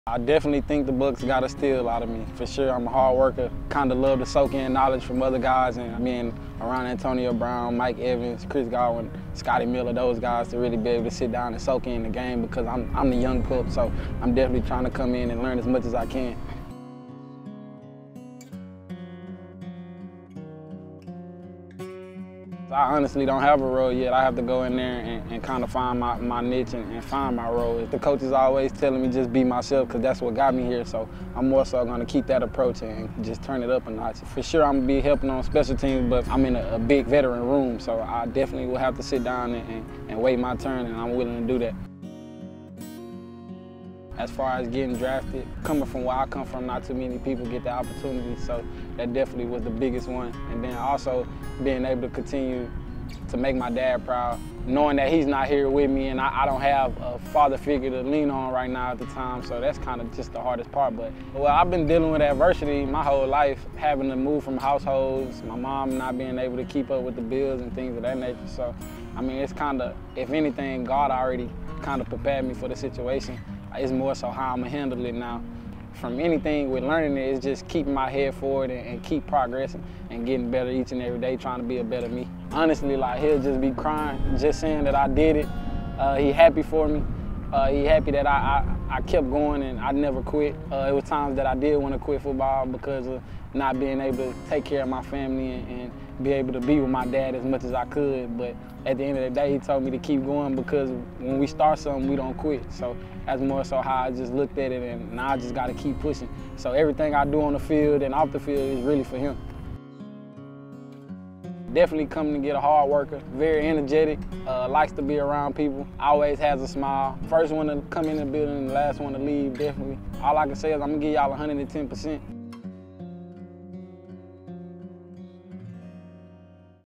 Jaelon Darden on Learning From Mike Evans, Chris Godwin & Antonio Brown | Meet the Rookies
Buccaneers wide receiver Jaelon Darden on his goals for his rookie season.